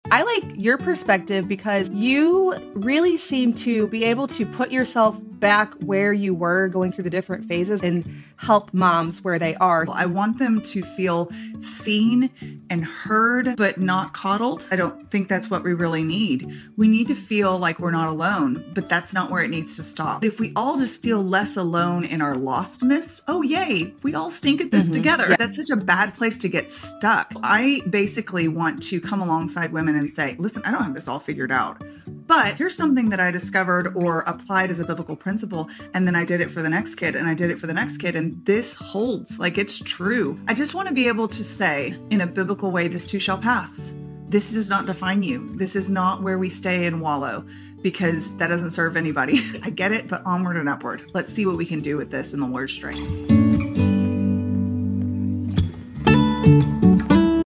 What moms really need to hear with Abbie of M is for Mama
Abbie of @m.is.for.mama.official is such an encouraging voice for today’s mother! This podcast episode is full of countless nuggets of truth and wisdom from a mama of 10. Find the Simple Farmhouse Life podcast on YouTube or your podcast app!

In this episode, we cover:
- All about Abbie’s 45 day trip to Europe with her 10 kids
- Feeling less alone in motherhood and moving forward out of the difficult places
- Finding freedom from the shame of not being able to keep up with everything on your plate
- Shifting your mindset around being interrupted by kids when trying to get things done
- Adjusting your expectations about what your house should look like
- Taking appropriate responsibility in parenting without succumbing to fear and control
- Keeping perspective about your capacity in different seasons of motherhood
- Avoiding the trap of comparing yourself to what everyone else is doing

Simple Farmhouse Life Podcast
197. Be a Countercultural Mom: Finding Joy, Peace, and Purpose in All Seasons of Motherhood | Abbie Halberstadt of M is for Mama